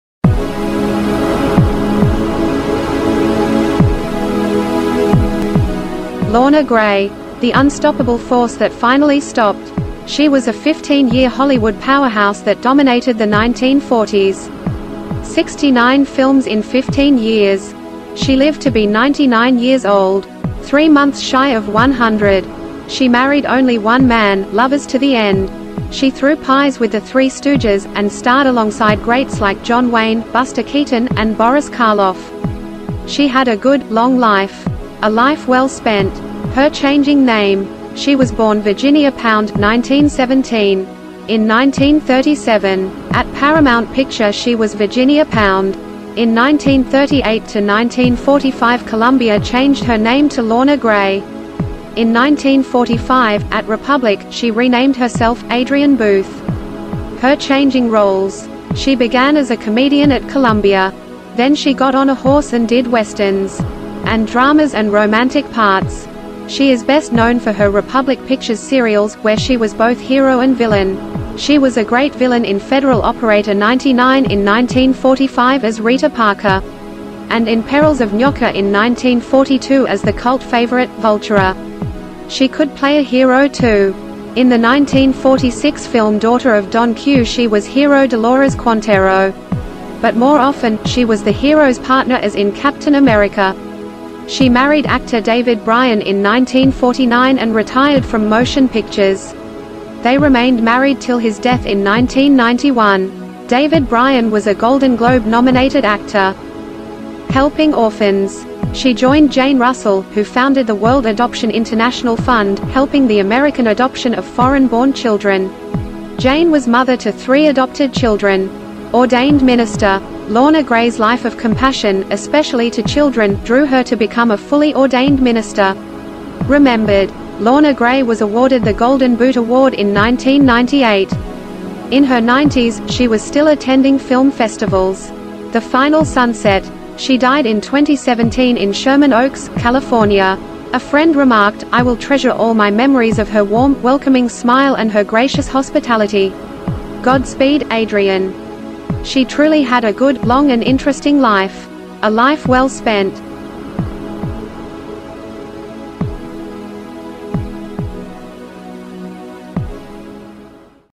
LORNA GREY Jungle Queen Turned Preacher
LORNA GRAY -THE UNSTOPPABLE FORCE
that finally stopped. She was a fifteen-year powerhouse that dominated the 1940s.

69 films in 15 years

She lived to be 99 years old. Three months shy of 100.

She married only one man—lovers to the end.

She lived ninety-nine years, threw pies with The Three Stooges, and starred alongside greats like John Wayne, Buster Keaton, and Boris Karloff. That is a good, long life -- well-spent!

HER CHANGING NAME
She was born Virginia Pound (1917)
1937. At Paramount Picture she was Virginia Pound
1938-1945 Columbia changed her name to Lorna Grey
1945-forward. At Republic, she renamed herself, Adrian Booth

HER CHANGING ROLES
She began as a comedian at Columbia.
Then she got on a horse and did Westerns.
And dramas and romantic parts.
She is best known for her Republic Pictures serials, where she was both hero and villain.
She was a great villain in Federal Operator 99 in 1945 as Rita Parker. And in Perils of Nyoka in 1942 as the cult favorite.Vultura.
She could play a hero too.
In the 1946 film Daughter of Don Q she was hero Dolores Quantero. But more often, she was the hero's partner as in Captain America.
She married actor David Brian in 1949 and retired from motion pictures. They remained married till his death in 1991. David Brian was a Golden Globe-nominated actor.

Helping Orphans
She Joined Jane Russell, who founded the World Adoption International Fund, helping the American adoption of foreign-born children. Jane was mother to three adopted children.
Ordained Minister
Lorna Gray's life of compassion, especially to children, drew her to become a fully ordained minister.

Remembered
Lorna Gray was awarded the Golden Boot Award in 1998.
In her nineties, she was still attending film festivals
The Final Sunset
She died in 2017 in Sherman Oaks, California. A friend remarked, "I will treasure all my memories of her warm, welcoming smile and her gracious hospitality. Godspeed, Adrian!"
Released into the Public Domain (CC-0)